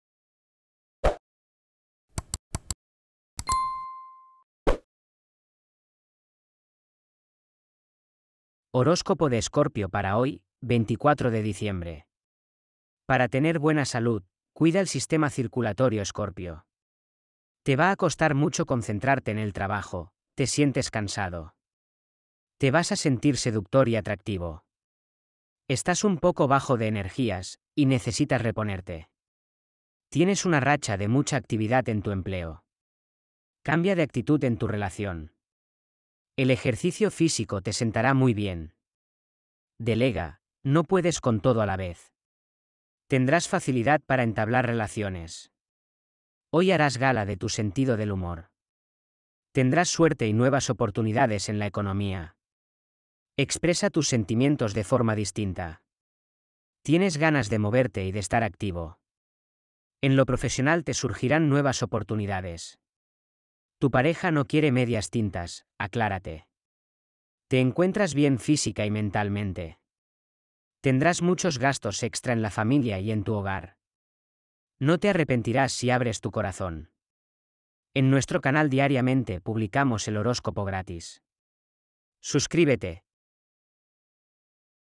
HORÓSCOPO DIARIO ⭐❤️💘 ESCORPIO ♏ VIERNES, 24 DE DICIEMBRE DE 2021
Horóscopo de hoy, viernes 24 de Diciembre de 2021. El horóscopo para hoy: descubre tu futuro gracias a mi horóscopo diario para ti.